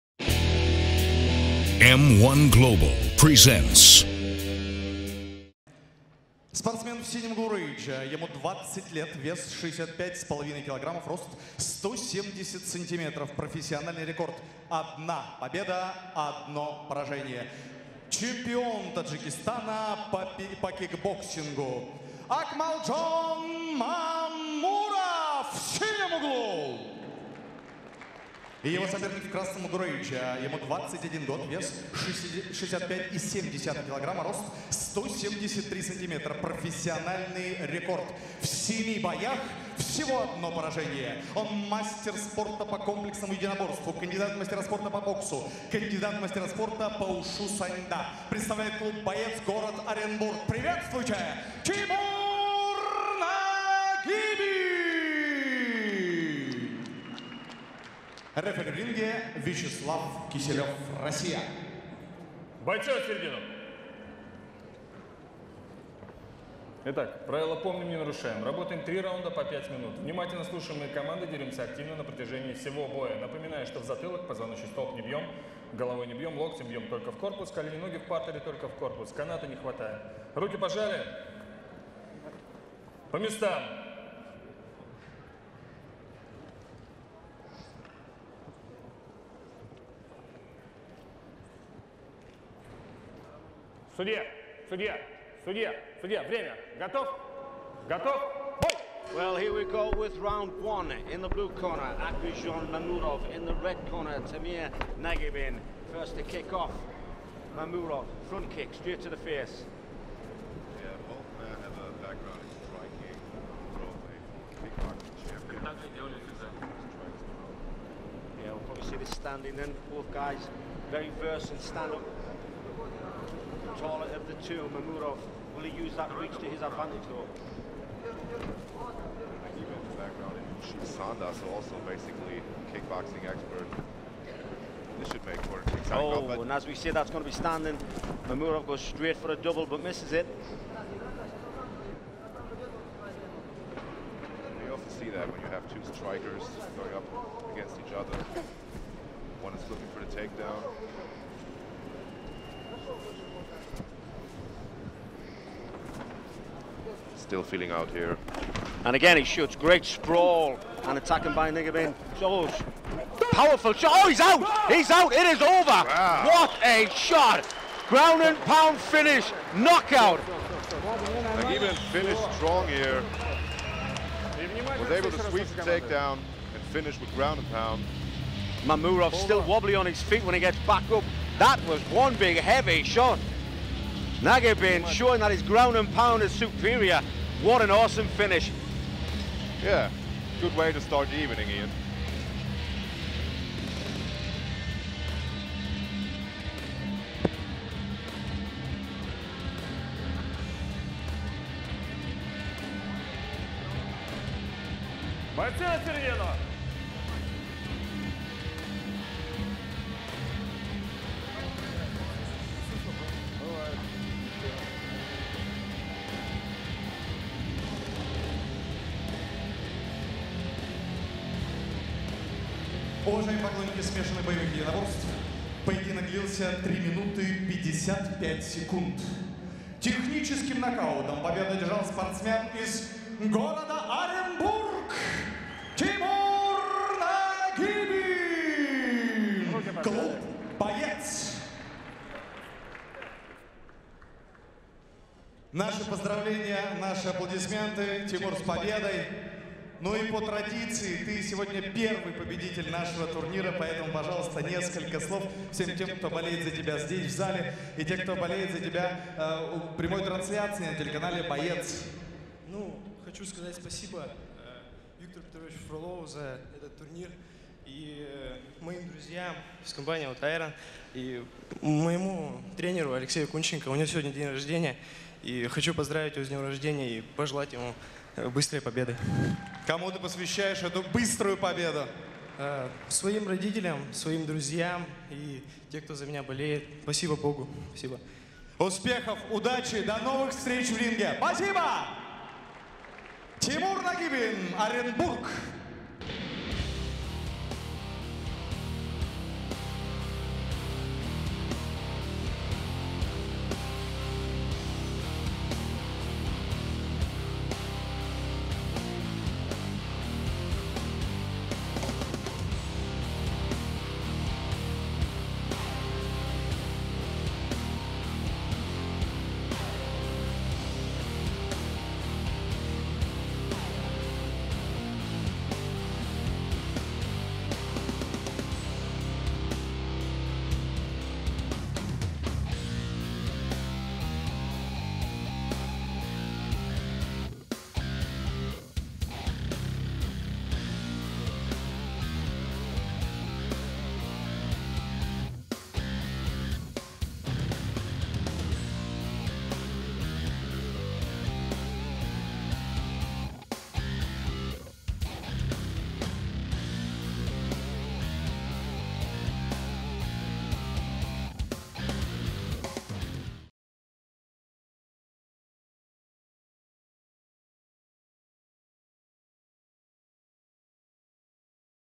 Timur Nagibin vs. Akmaldzhon Mamurov, M-1 Challenge 57, Orenburg | Full fight - FREE
Featherweight fight between Timur Nagibin, Russia, club "Fighter" and Akmaldzhon Mamurov from Tajikistan. M-1 Challenge 57 was held in Orenburg, Russia,  2nd May 2015.